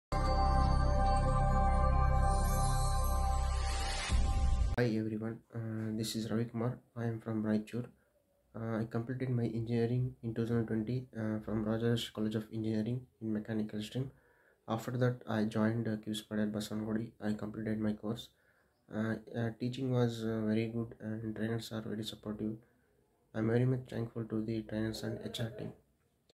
The Best Software Training Institute In India | QSPIDERS BASAVANAGUDI
Our Student RAVI KUMAR  giving feedback about the Institution, Trainers, HRs and his Experience in QSpiders Basavanagudi.